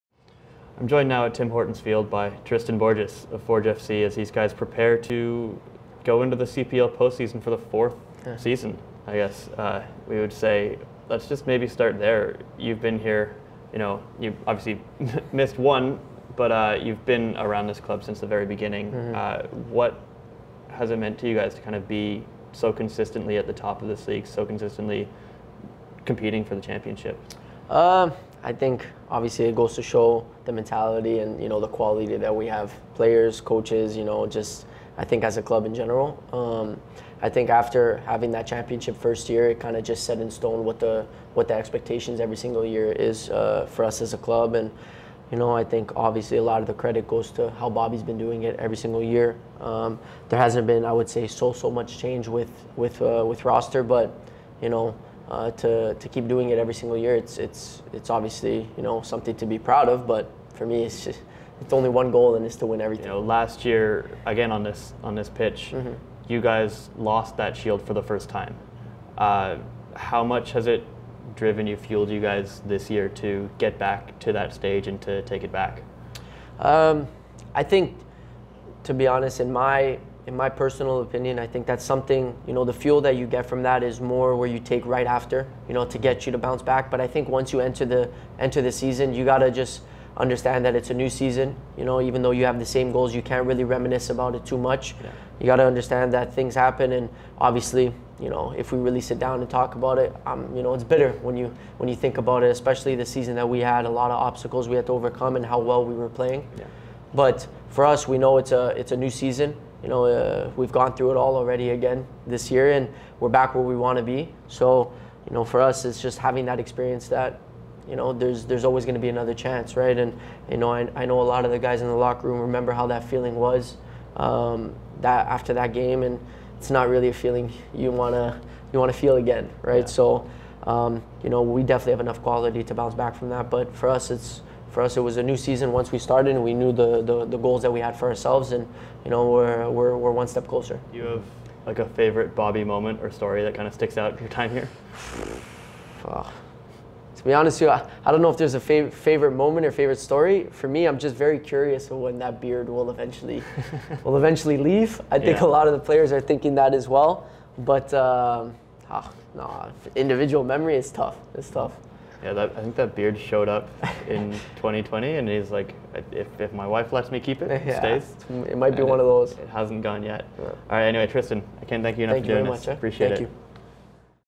2022 Playoff Previews: Forge FC's Tristan Borges speaks to Charlie O'Connor-Clarke
"After having that championship in the first year, it just set in stone what the expectation every year is for us as a club." As Forge Football Club take part in their fourth CPL playoffs in a row, Tristan Borges spoke to Charlie O'Connor-Clarke 🗣️